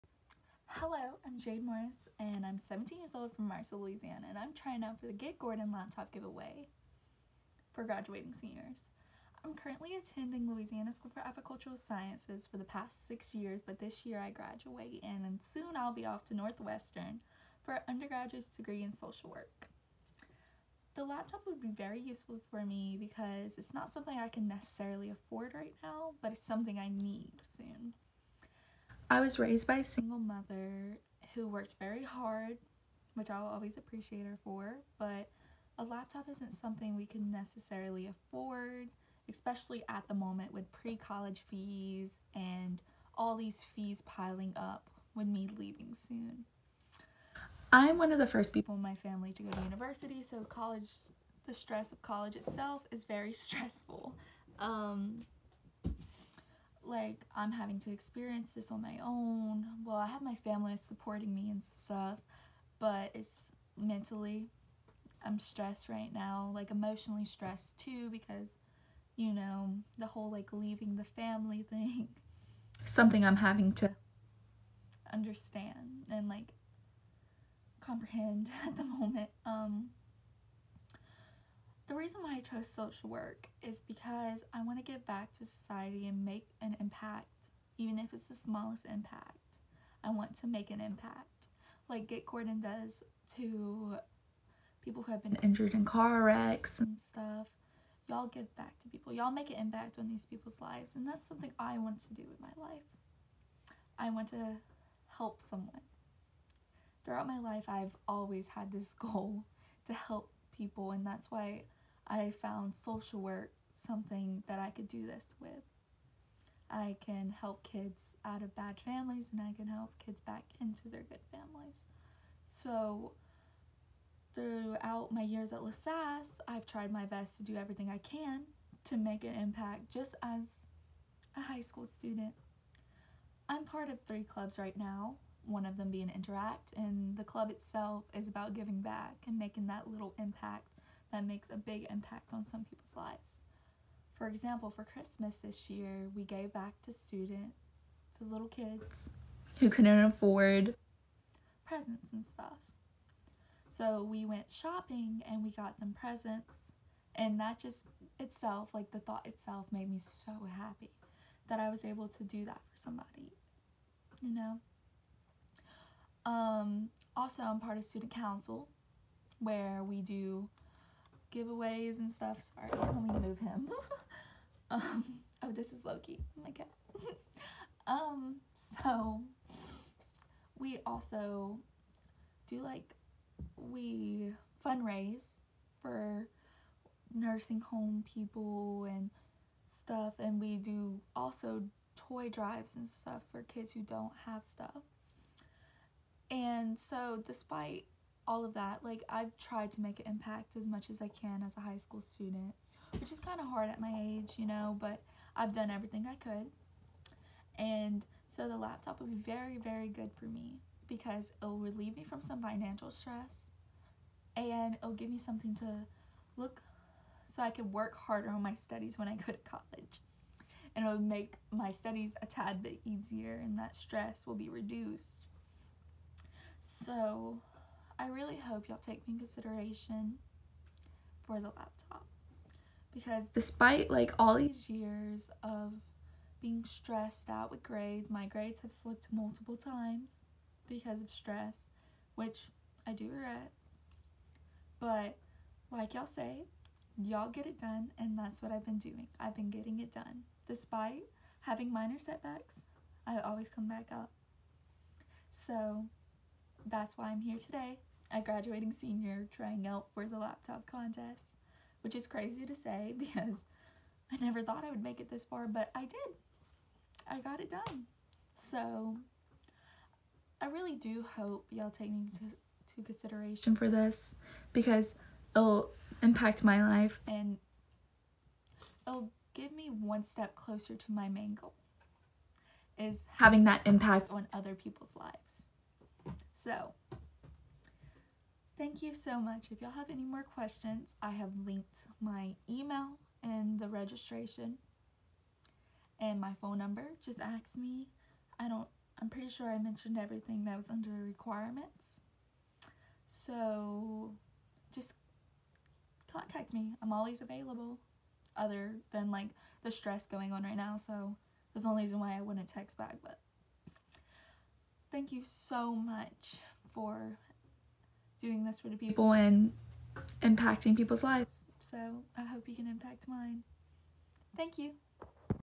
Get Gordon Laptop Giveaway Entry- Jade Morris
Sorry for the audio issues! And the slight interruption from my cat😂  I recorded this video as soon as I saw the commercial, so that’s why I look how I do. Hopefully y’all can take me into consideration! Thank you :)